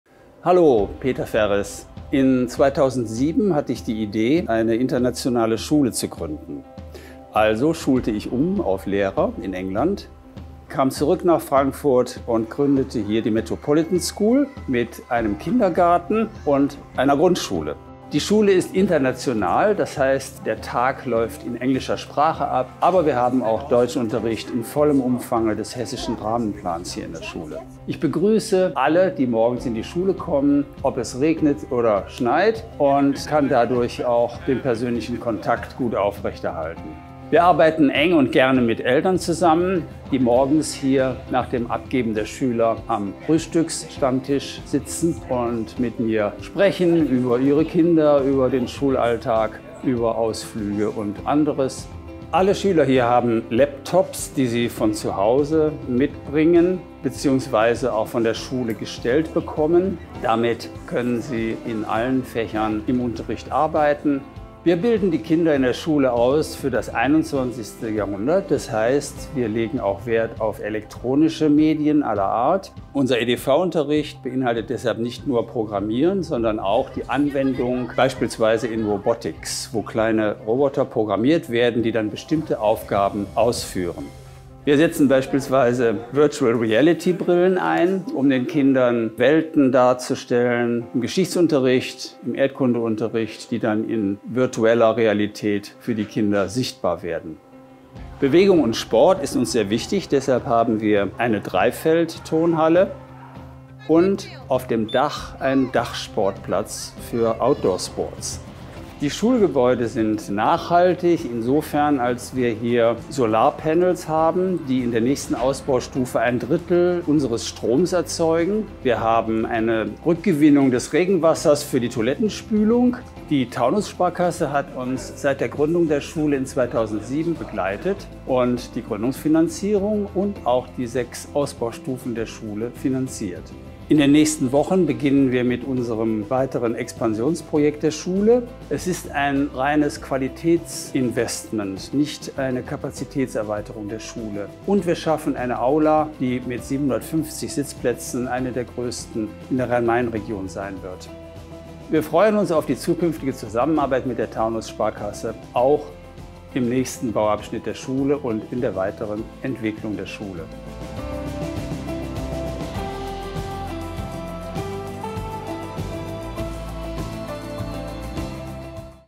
Sparkasse: Metropolitan School Frankfurt
Thank you to Sparkasse for supporting Metropolitan School Frankfurt projects and for creating this beautiful video to show our school!